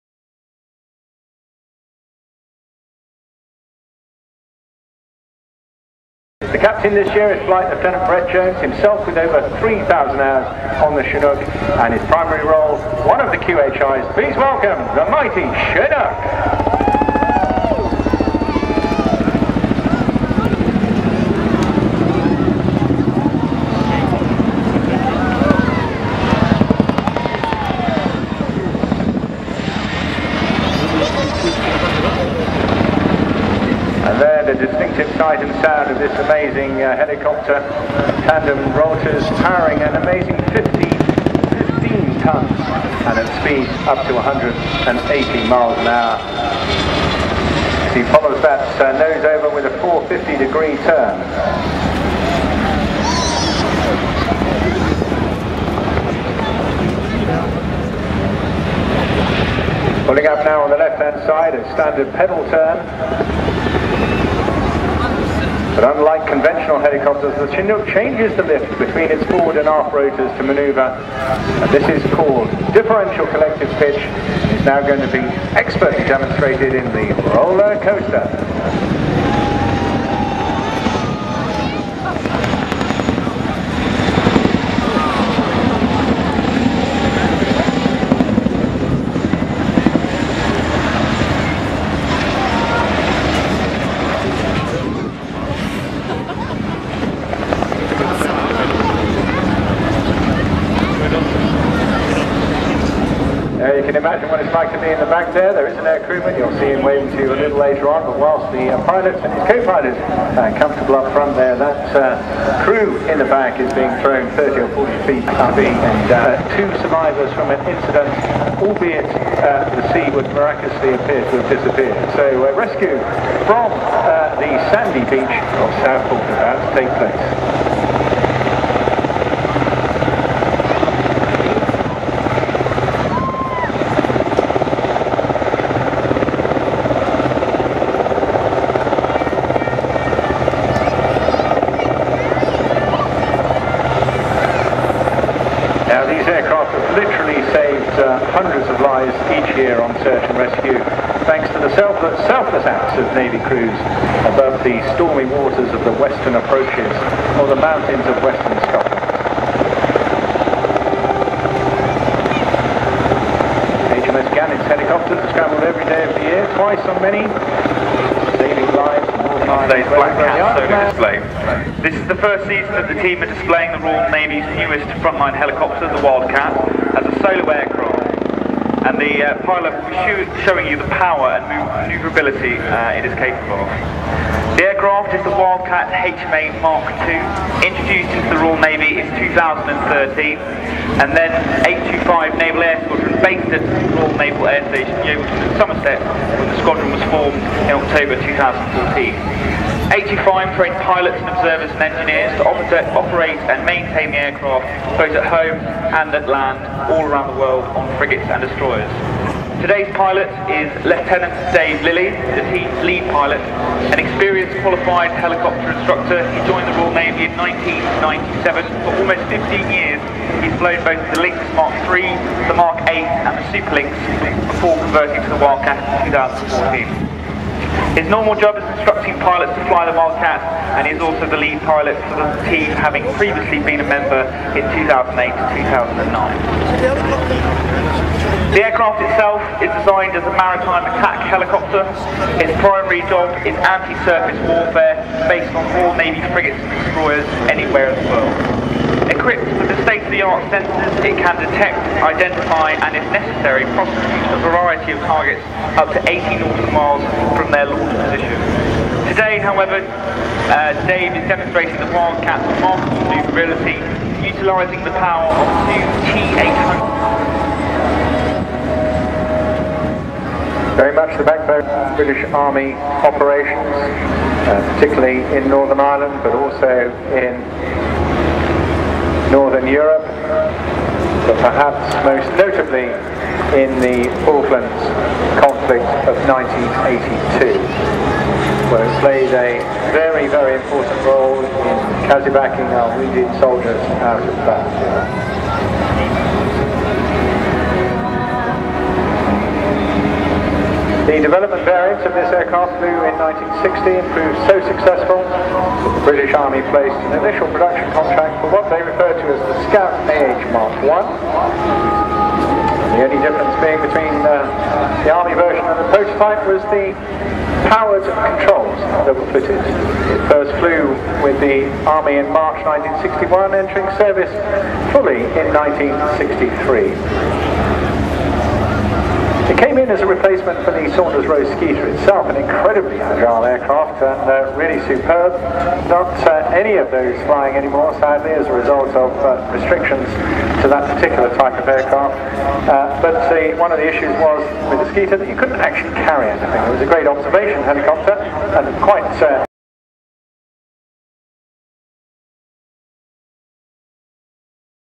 Southport Air Show Helicopters 2015
4 helicopters on display at the Southport Air Show on 19 September 2015.  Chinook, Sea King, Wildcat and Scout